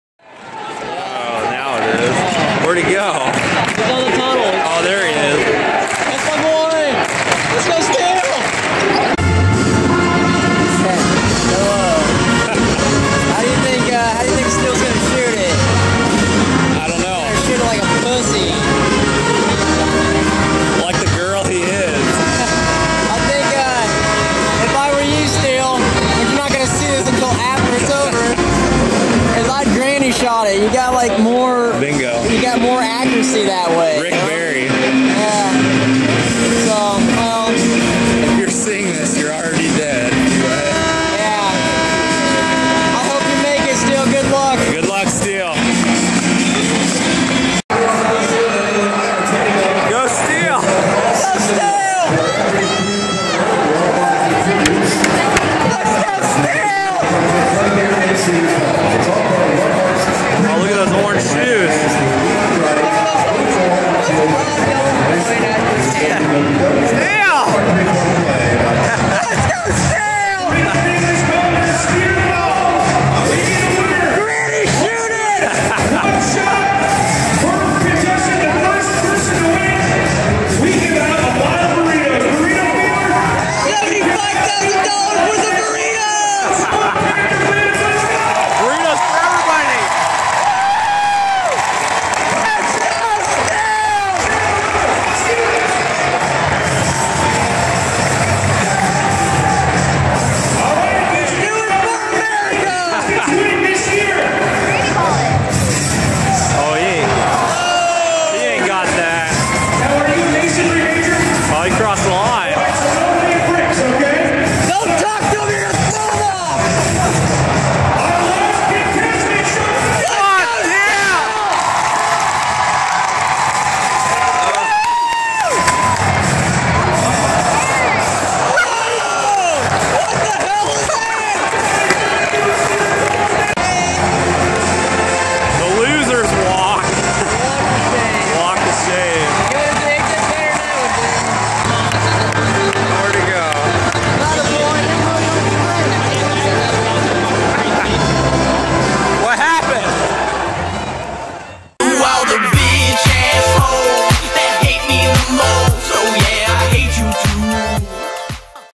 Steele's Half Court Shot for Burritos
Steele unleashes the beast as he shoots a half court shot for burritos and his life.